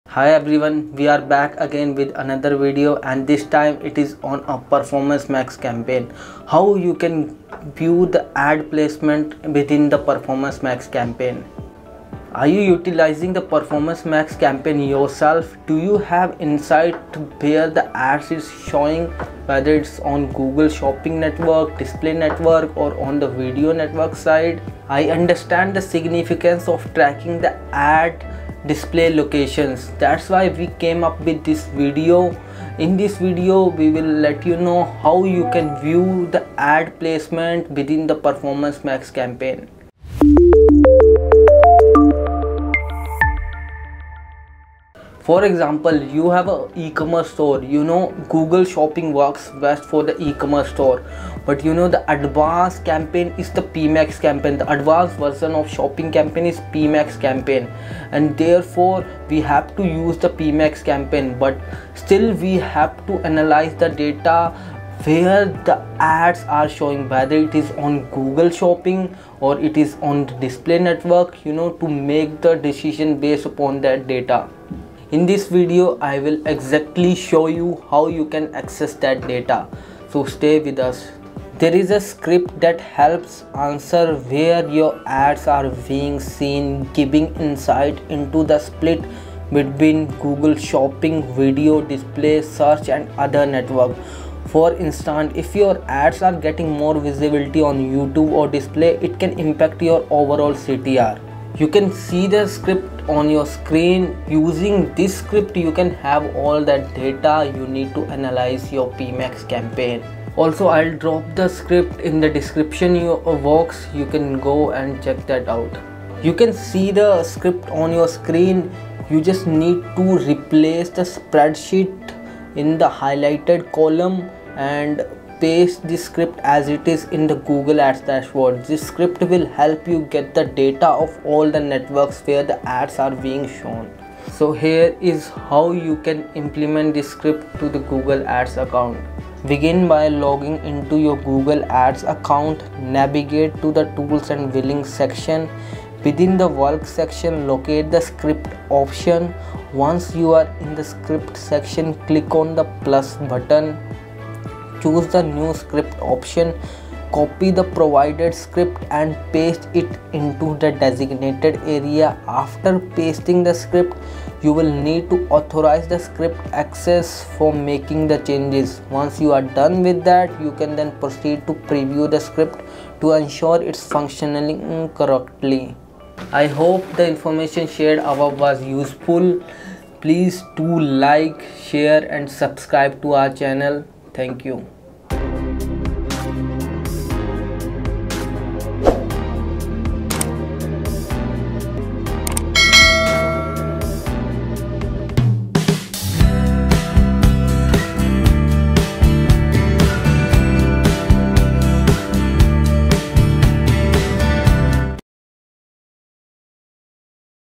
You Can Now See AD PLACEMENT in Performance Max Campaign - Pmax Optimization Tips
In this video, we'll explore how do you see ad placements of your Performance Max campaign on Google AdWords.

⏱️TIMESTAMPS⏱️

0:00 Introduction
0:30 View Ad Placemments
0:45 E-Commerce Store Example
1:21 View Script Now
2:14 Implementation of Script
2:48 Like, Share & Subscribe

Resource Link

Hi everyone! 🎥 We're here with another video, and this time, we're diving into the world of Performance Max campaigns. 🚀 Ever wondered where your ads are showing within a Performance Max campaign? 🌐 Are you using this campaign type and curious about its placements? Well, tracking your ad display locations is crucial. That's why we've created this video! 🎯

In this video, we'll show you exactly how to view the ad placement within a Performance Max campaign. Let's say you run an eCommerce store, and you know Google Shopping is a powerhouse. But the advanced campaign to explore is the Performance Max (PMax) campaign. 🛍️✨ However, you still need to analyze where your ads are showing—whether it's on Google Shopping, the display network, or elsewhere. Your decisions can be data-driven.

💡 We've got a script to help answer the question: Where are your ads being seen? This script provides insights into the distribution between Google Shopping, video display, search, and other networks. Knowing if your ads get more visibility on YouTube or the display network can impact your overall Click-Through Rate (CTR).

📜 You can see the script on your screen right now. You only need to replace the spreadsheet in the highlighted column and paste this script as it is in Google Ads. This script will gather all the data you need to analyze your PMax campaign.

📁 Also, we'll drop the script in the description for your convenience. Feel free to check it out there!

🔧 Here's how to implement this script in your Google Ads account:

- Start by logging into your Google Ads account.
- Navigate to the Tools and Settings section.
- Within the Tools section, look for the Script option.
- Once in the Script section, click the plus button, choose the new script option, and then copy the provided script. Paste it into the designated area.
- After pasting the script, you'll need to authorize script access for making changes.
- Once that's done, you can proceed to preview the script to ensure it functions correctly.